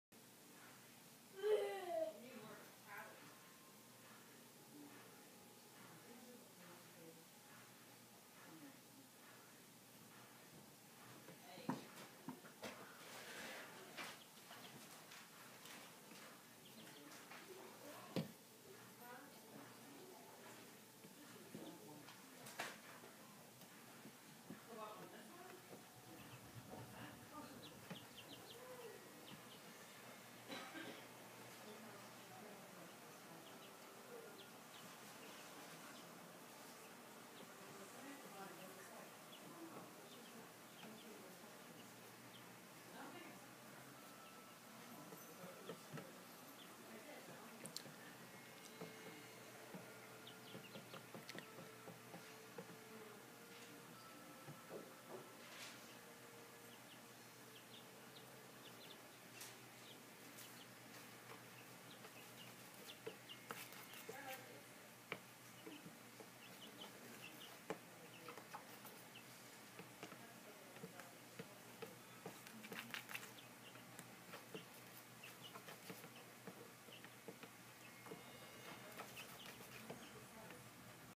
The Chickens Acting All Chickeny
My 2 week old chicks.